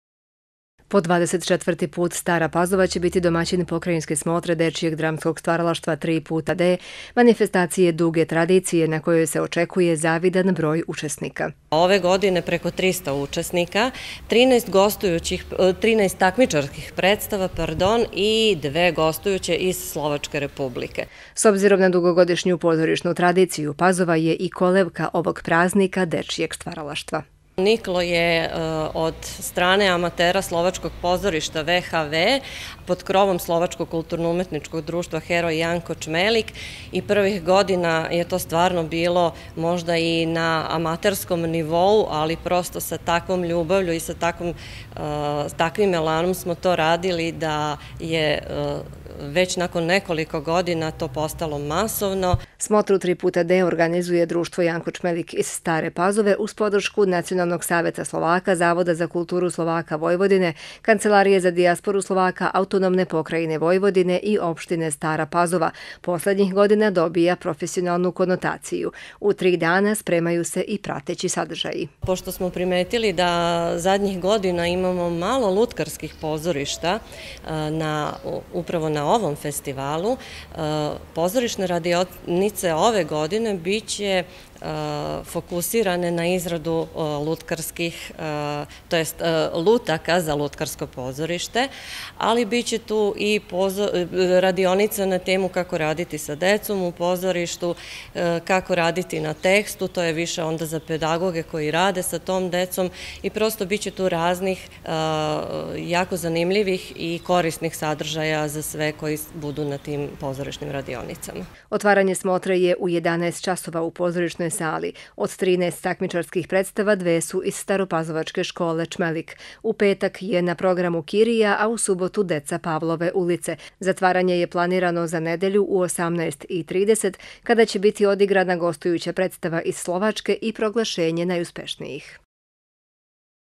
Najava festivala 3xD
U Staroj Pazovi će od petka do nedelje biti održana 24. smotra dečjeg dramskog stvaralaštva Slovaka u Vojvodini. Prijavljeno je 13 takmičarskih, i dve gostujuće predstave koje stižu iz Slovačke, a u okviru tri festivalska dana biće organizovane i radionioce o radu sa decom u pozorištu i izradi lutaka. Svečano otvaranje je sutra u 11 časova u pozorišnoj Sali, a domaćini imaju dve predstave u konkukurenciji: Deca Pavlove ulice i Kirija, OŠ Čmelik.